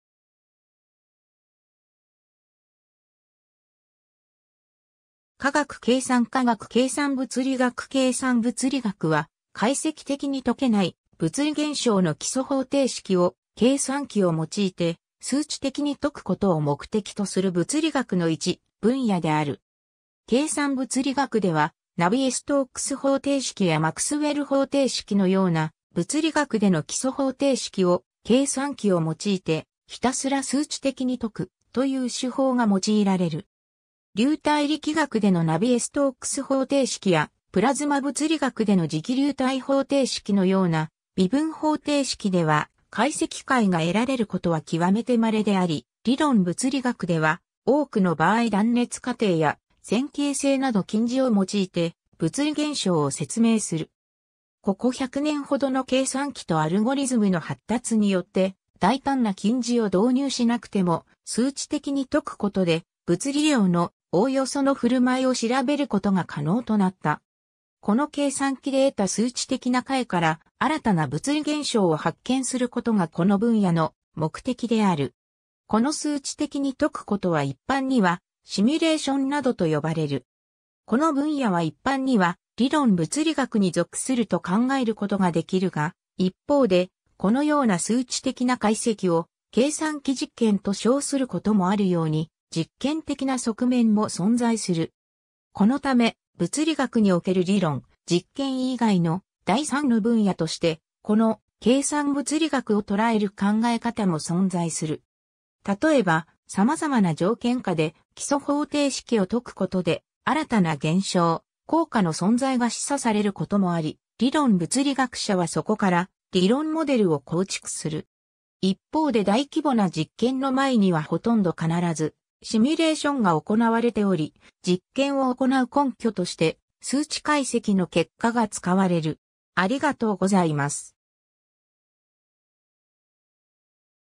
計算物理学
科学 」 計算科学 」 計算物理学 計算物理学（けいさんぶつりがく、英語: computational physics）は、解析的に解けない物理現象の基礎方程式を計算機（コンピュータ）を用いて数値的に解くことを目的とする物理学の一分野である。
 計算物理学では、ナヴィエ・ストークス方程式やマクスウェル方程式のような物理学での基礎方程式を計算機を用いてひたすら数値的に解く、という手法が用いられる。
 流体力学でのナヴィエ・ストークス方程式やプラズマ物理学での磁気流体方程式のような微分方程式では、解析解が得られることはきわめてまれであり、理論物理学では多くの場合断熱過程や線形性など近似を用いて物理現象を説明する。
 ここ100年ほどの計算機とアルゴリズムの発達によって、大胆な近似を導入しなくても、「数値的に解く」ことで物理量のおおよその振る舞いを調べることが可能となった。
この計算機で得た数値的な解から新たな物理現象を発見することがこの分野の目的である。
この数値的に解くことは一般には「シミュレーション」などと呼ばれる。
 この分野は一般には、理論物理学に属すると考えることができるが、一方で、このような数値的な解析を「計算機実験」と称することもあるように、実験的な側面も存在する。
このため、物理学における理論、実験以外の第三の分野として、この「計算物理学」を捉える考え方も存在する。
たとえば、さまざまな条件下で基礎方程式を解くことで、新たな現象、効果の存在が示唆されることもあり、理論物理学者はそこから理論モデルを構築する。
一方で大規模な実験の前にはほとんど必ずシミュレーションが行われており、実験を行う根拠として数値解析の結果が使われる。